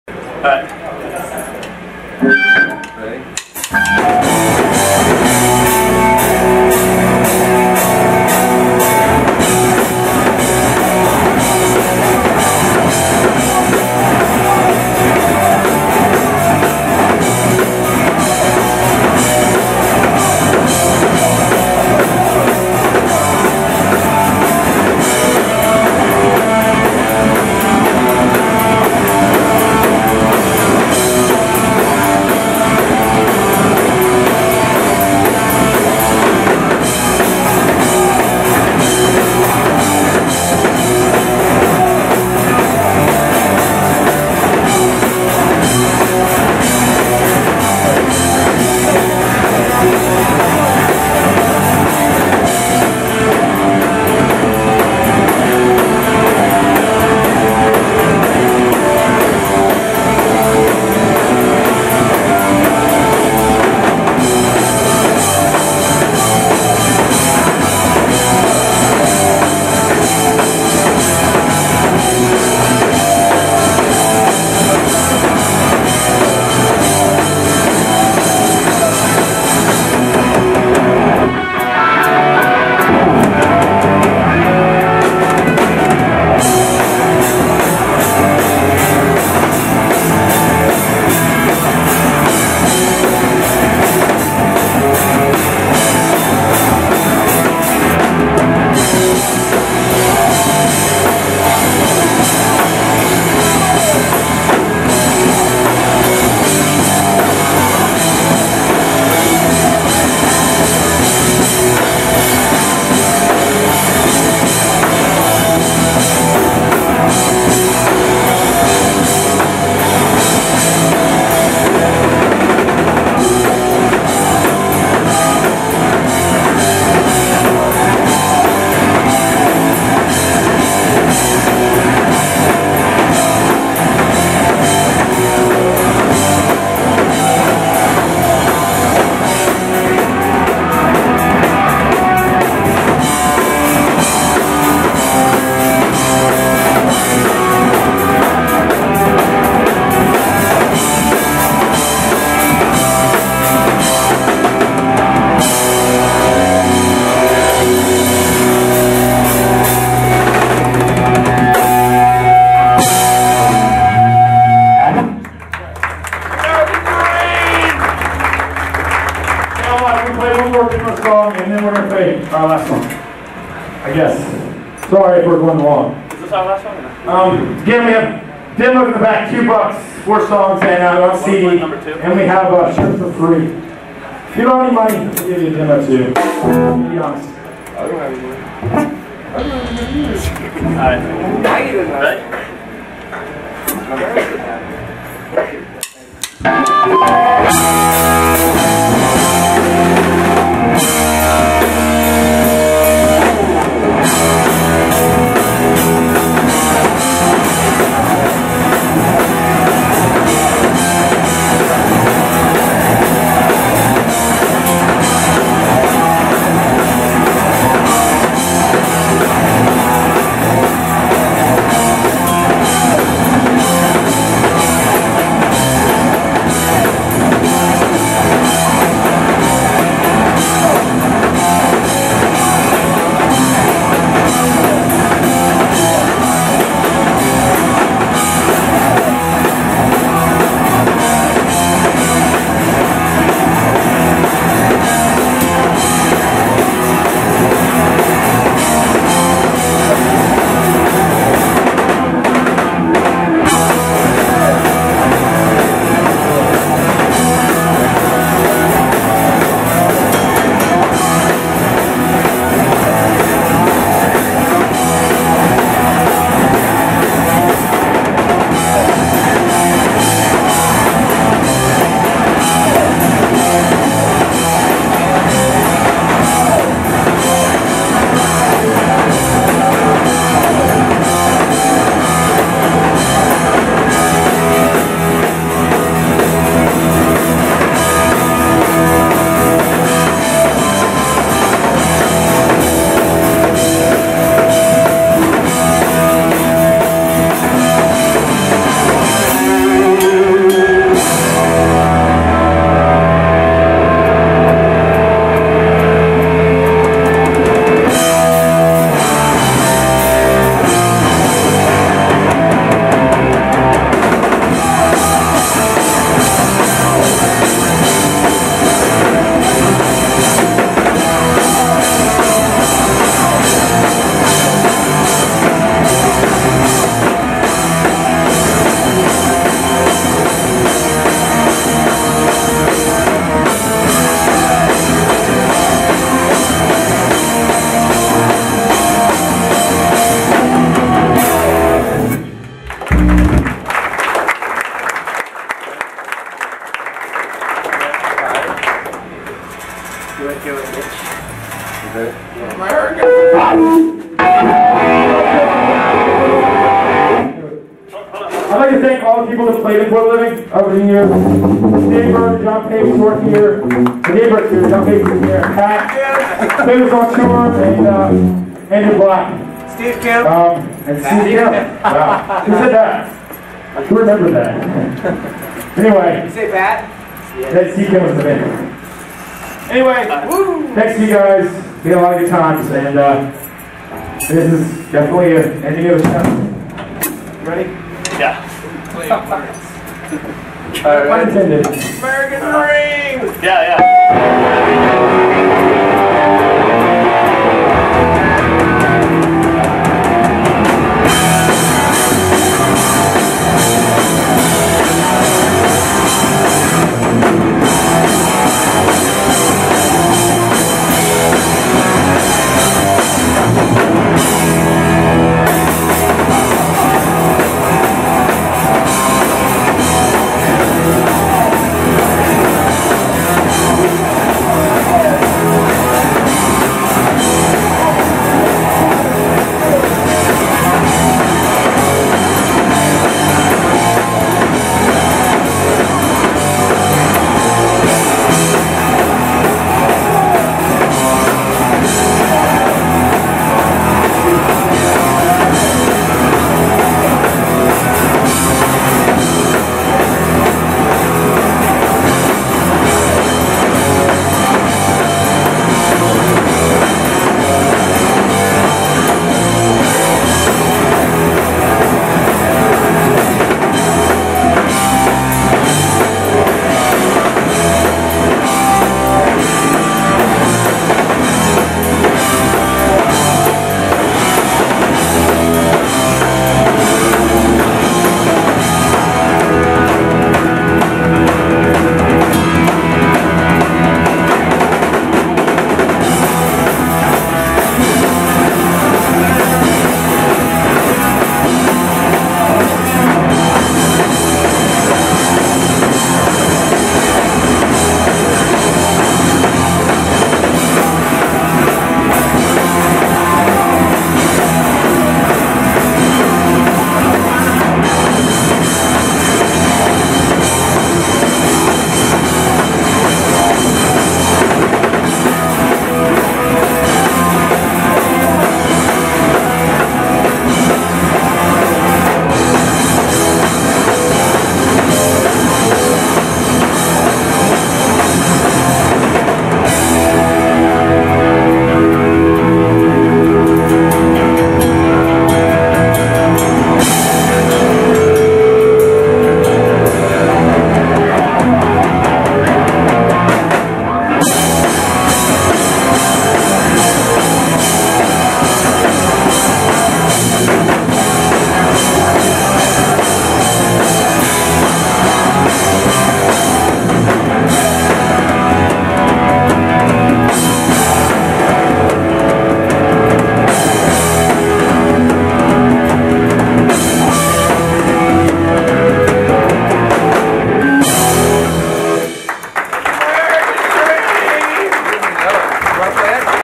Primary - U of MD Food Co-op '01
Primary (otherwise known as For The Living) Live @ The UofMD Food Co-op, College Park MD - 11 August 2001